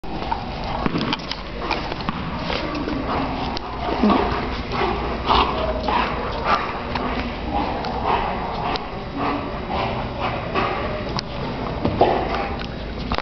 Meet Six a Shepherd currently available for adoption at Petango.com! 4/27/2012 11:53:26 AM
Busy Bee?
I'm a naturally playful, curious, and trusting canine. Take me for a big walk every day; give me something to do. After my job's done, I'll curl up in front of the fire with you in the evenings. (Externally motivated)